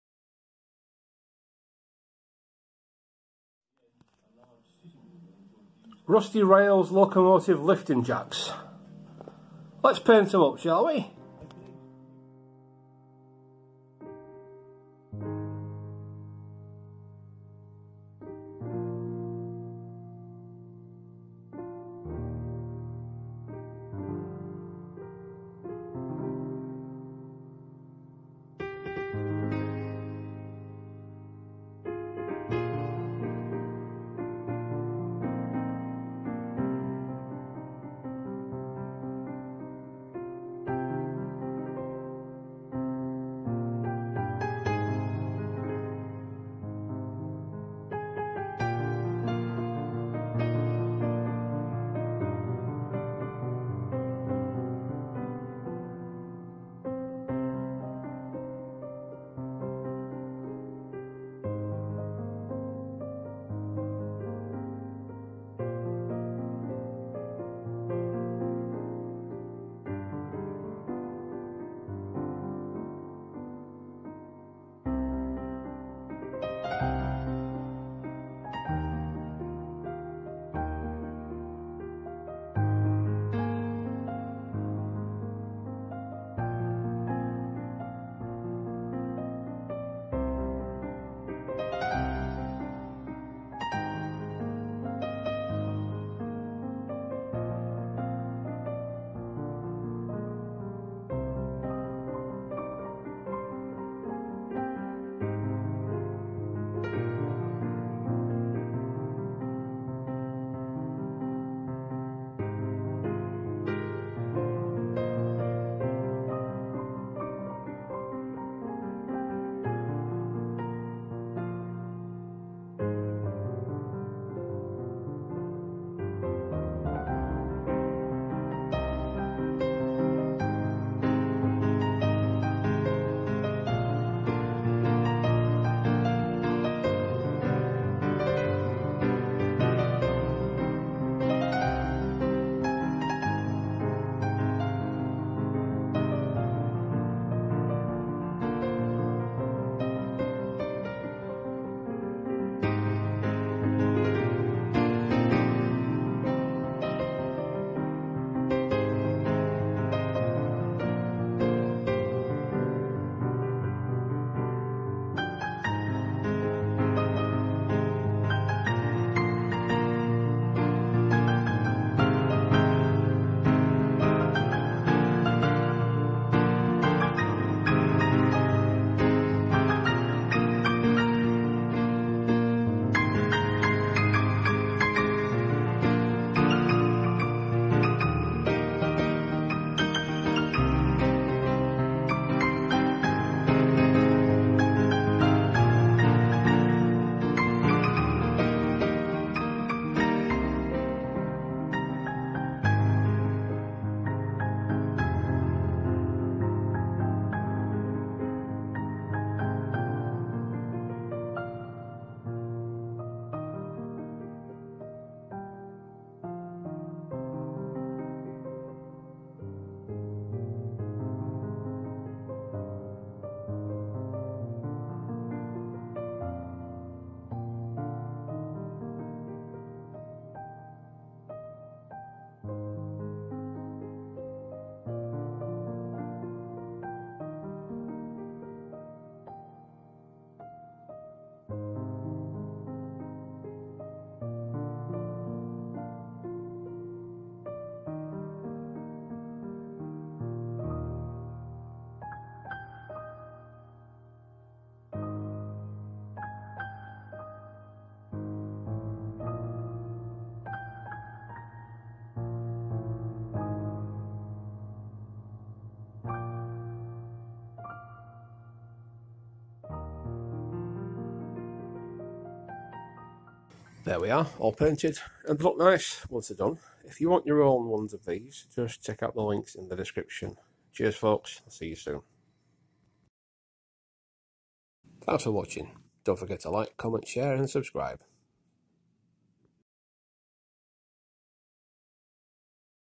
Painting Locomotive Jack Stands
Super detailed jack stands here, take paint lovely too!❤️😁
3D printing has CHANGED the game folks 👍